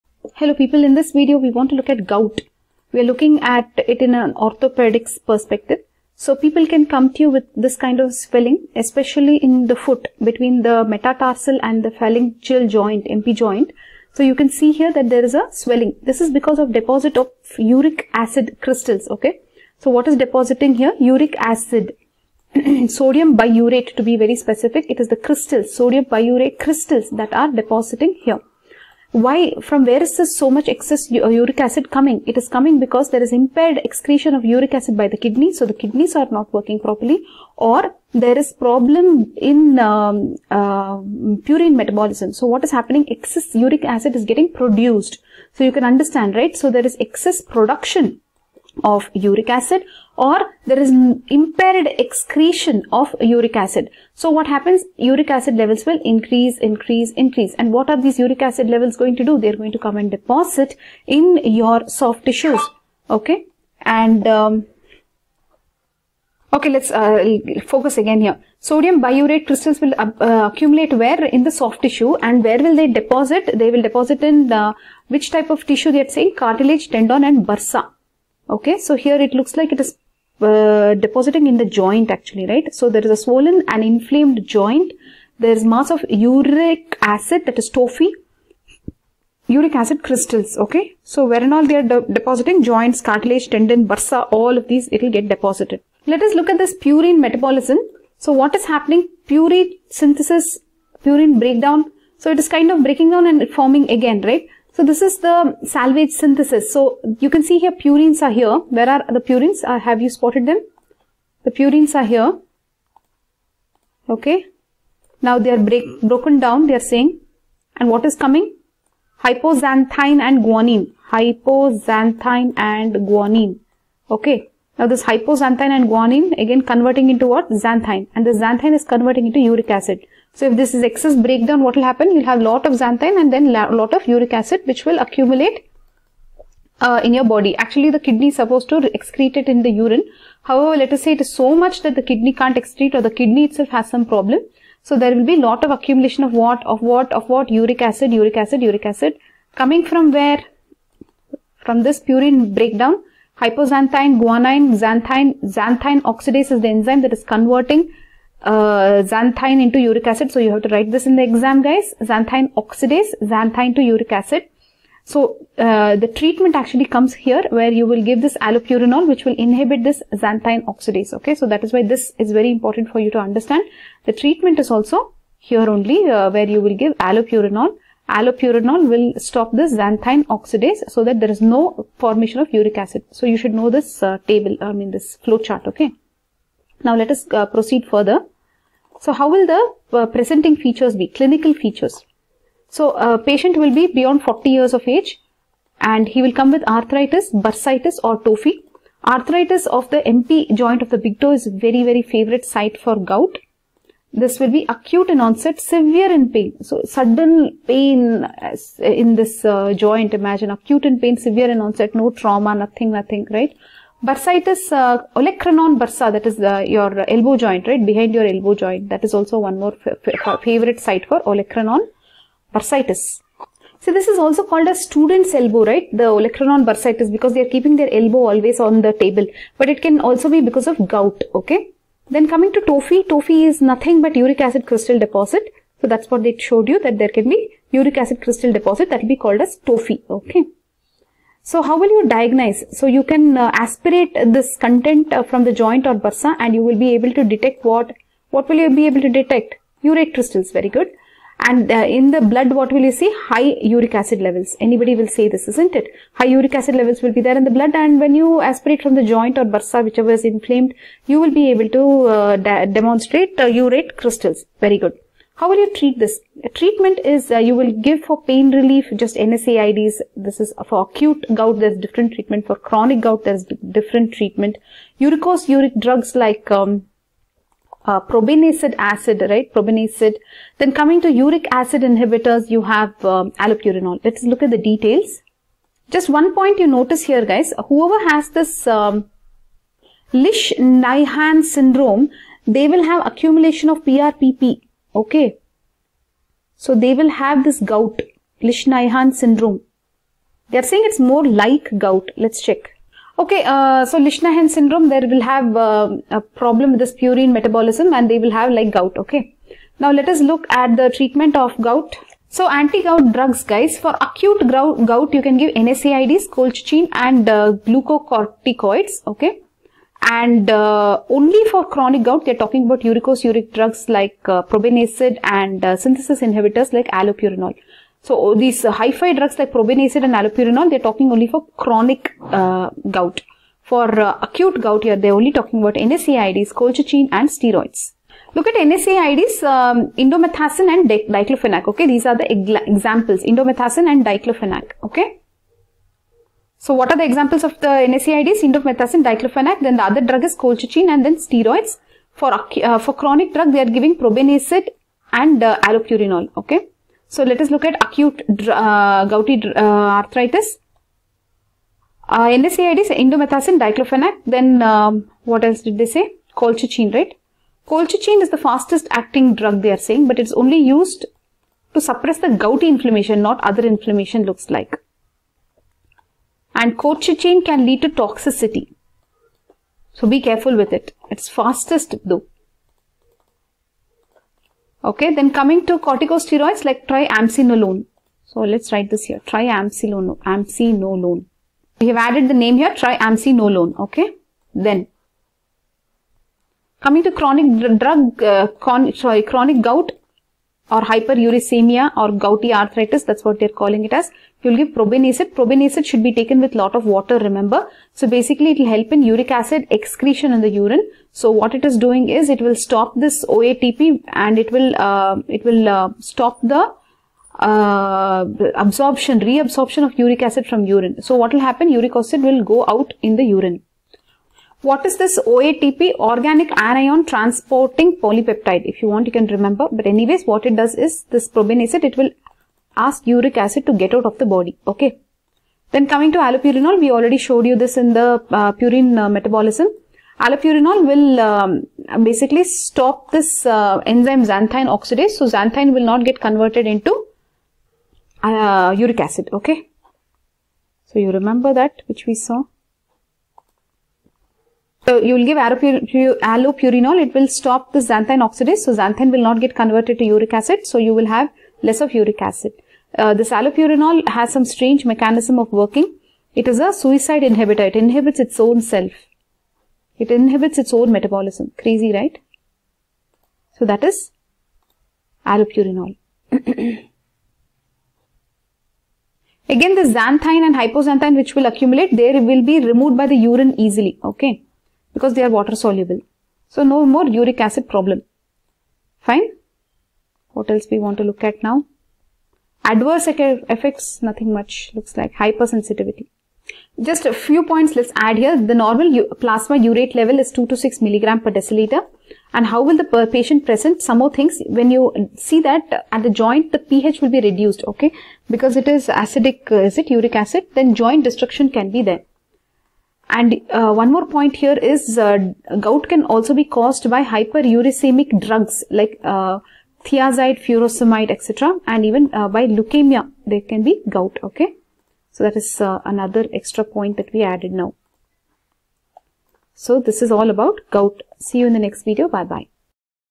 Surgery 312 Gout causes treatment clinical features hyperuricemia uricosuric drugs gouty arthritis
Normal urate level in blood 5mg/dL